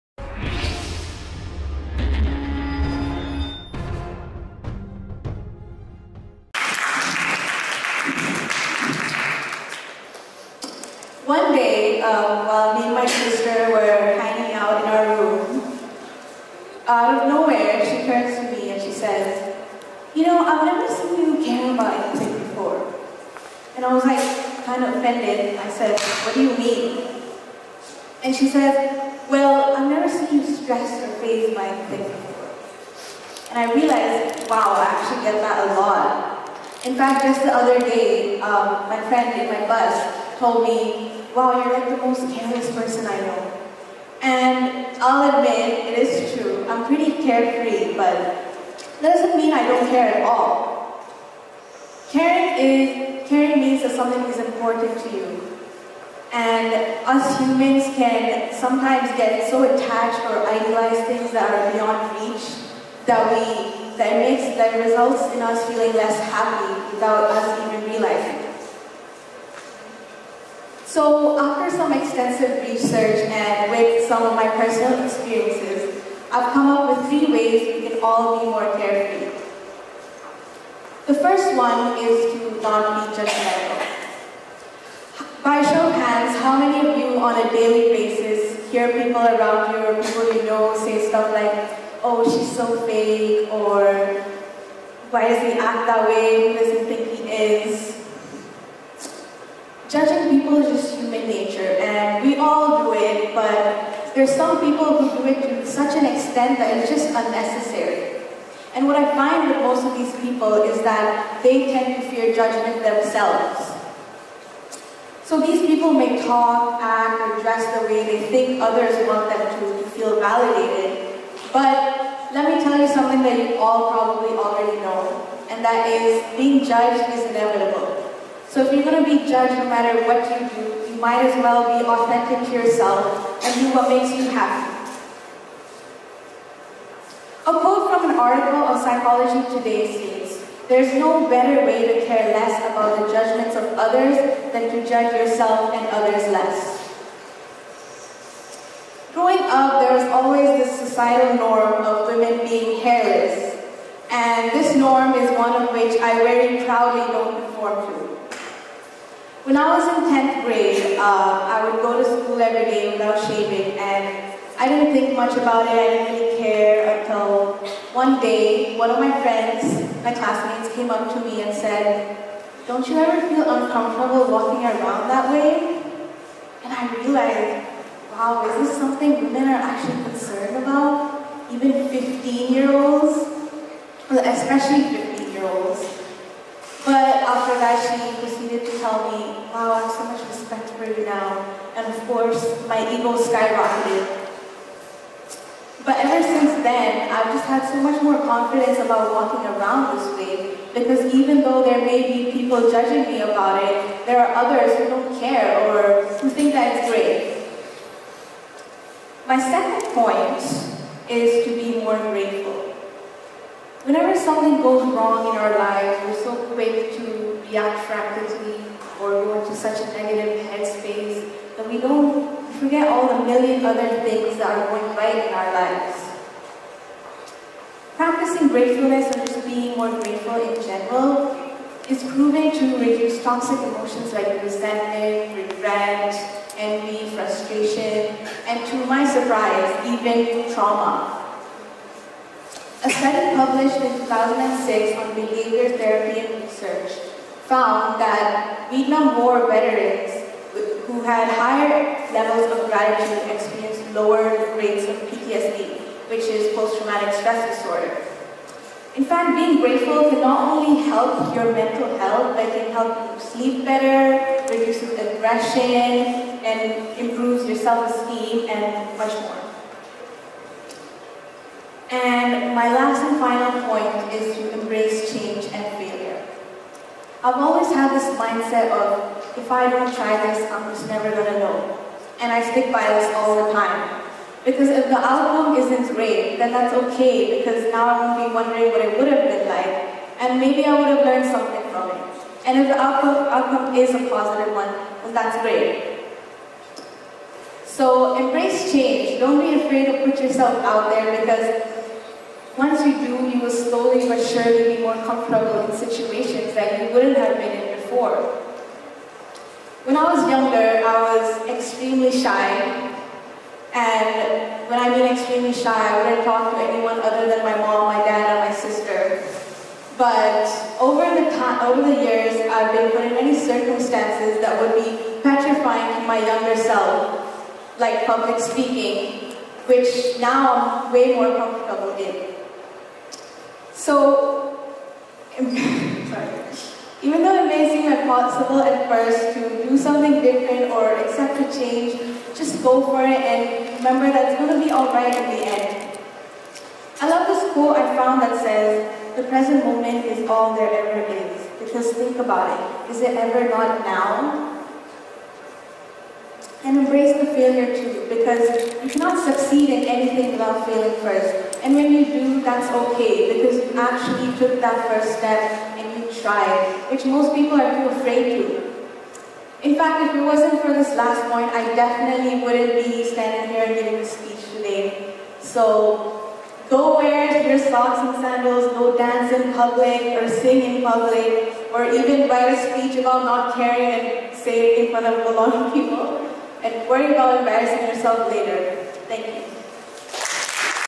A psychological take on how to be more carefree. | Arthi Srinivasan | TEDxYouth@TCHS
A psychological take on how to be more carefree.
Arthi is a year 13 student who is more interested in design and technology. She is also more passion about music and also a good public speaker. This talk was given at a TEDx event using the TED conference format but independently organized by a local community.